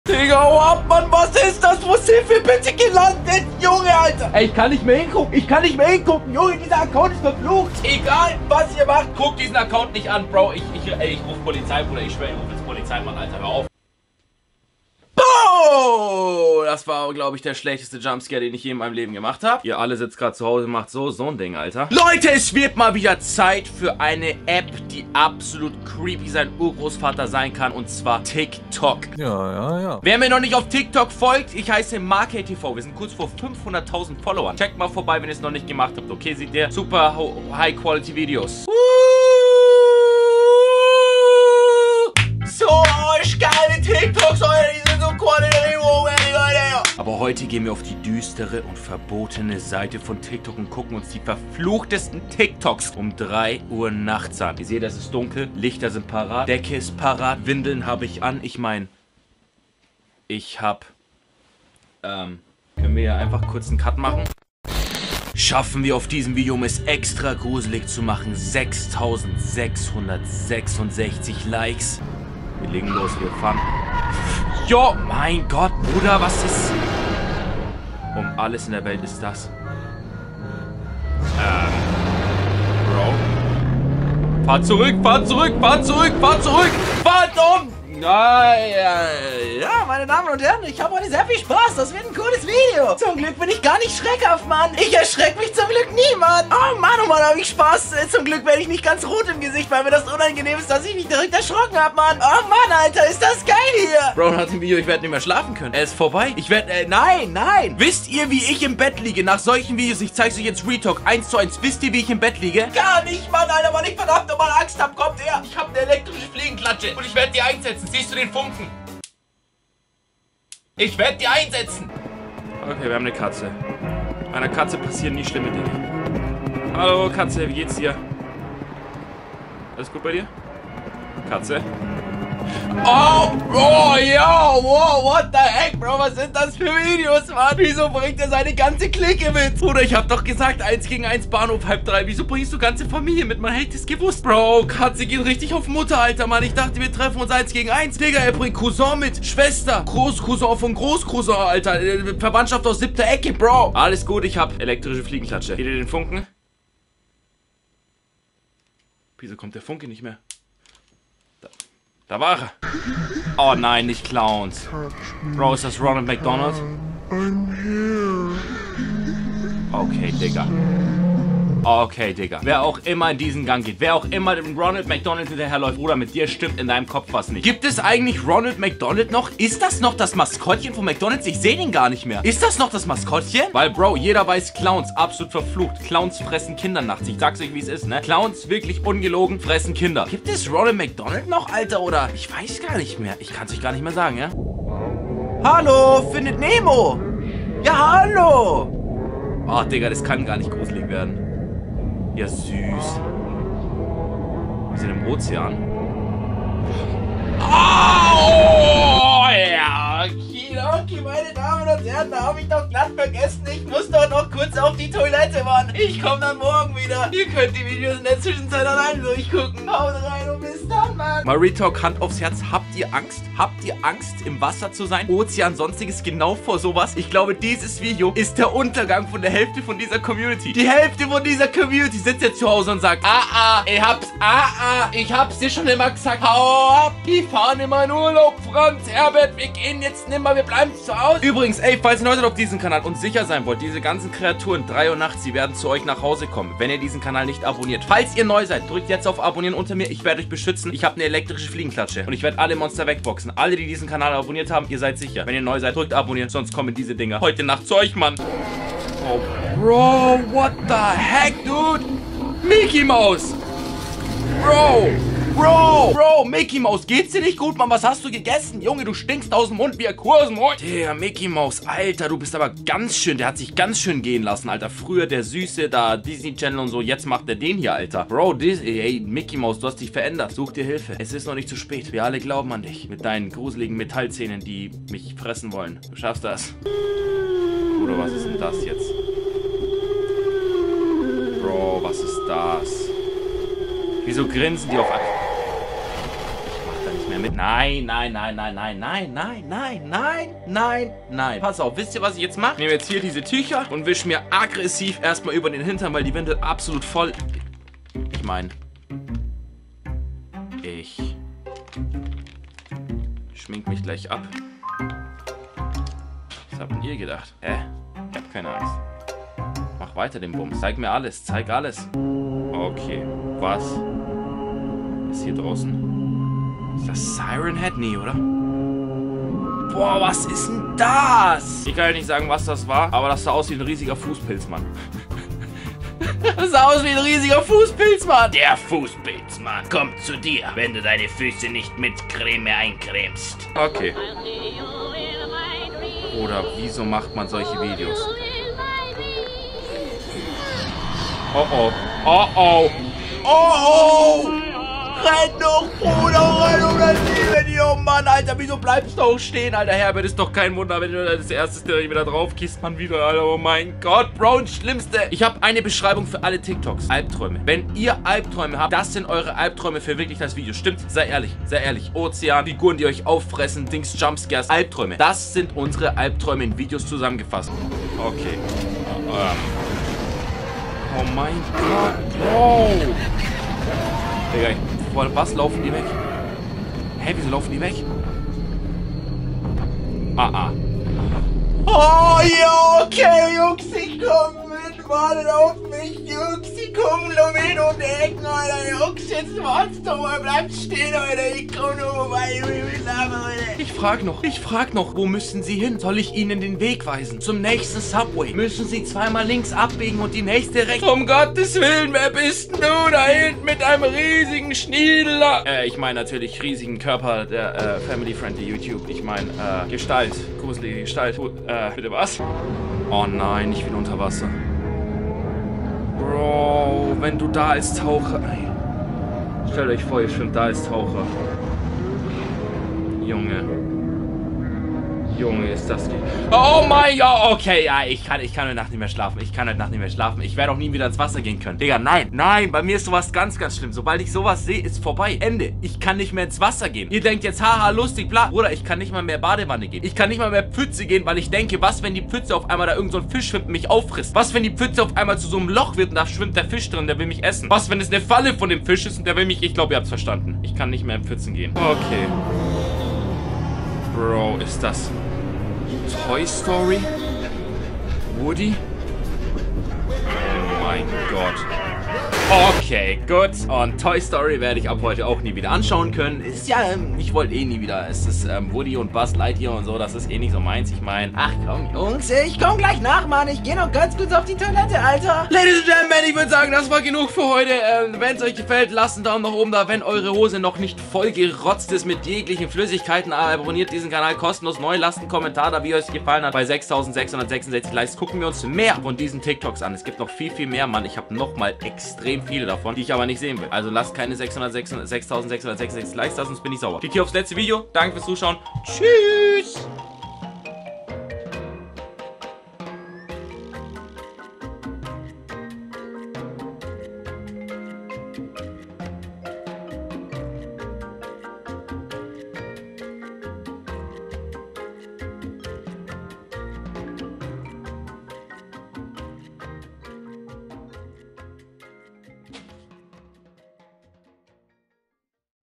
4 verfluchte ❌ TIKTOK ACCOUNTS die du niemals *DREI UHR NACHTS* alleine anschauen solltest (warnung)
5 verfluchte ❌ TIKTOK ACCOUNTS die du niemals *DREI UHR NACHTS* alleine anschauen solltest. Vorsicht gruselig 3 uhr nachts!

► Video liken

FERTIG!